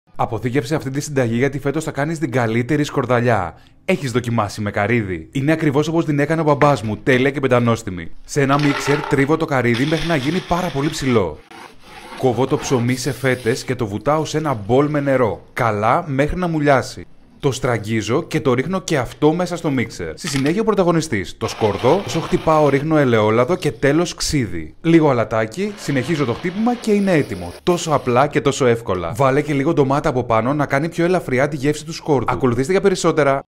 Σκορδαλιά με καρύδι | Petros Maounatzis
🔴  🔔Νέο βίντεο κάθε Τρίτη & Παρασκευή!

Υλικά:
- 100 γρ. αλεσμένο καρύδι
- 3 φέτες ψωμί
- 2 μεγάλες σκελίδες σκόρδο
- 70 γρ. ελαιόλαδο
- 100 γρ. ξύδι
- λίγο αλάτι